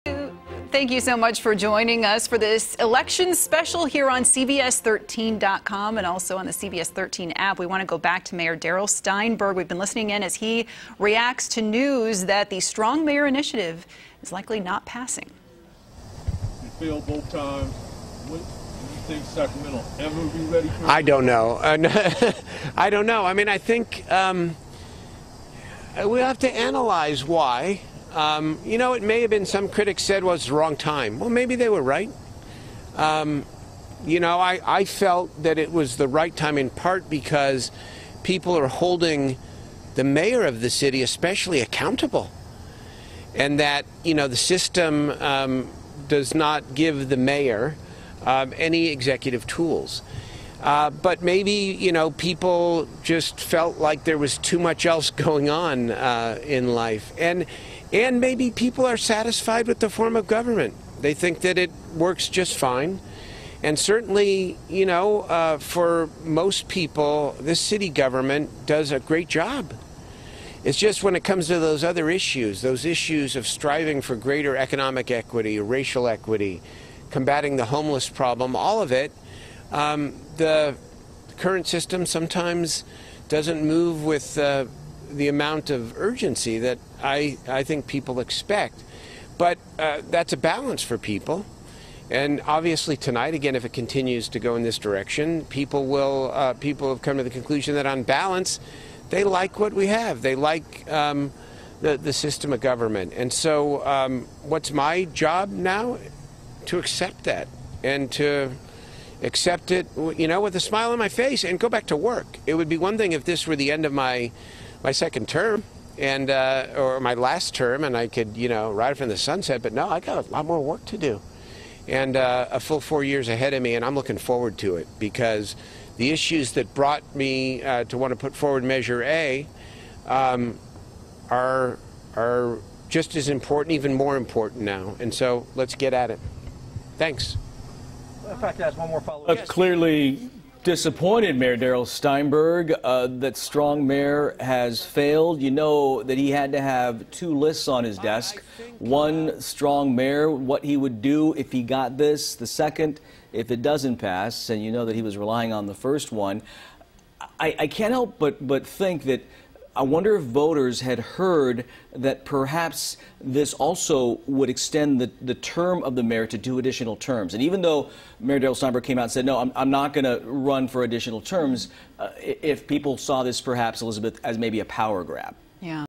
Sacramento Mayor Darrell Steinberg Talks About Trailing Strong Mayor Initiative
Mayor Steinberg spoke to CBS13 on November 3, 2020, about the strong mayor initiative, which seemed to be trailing.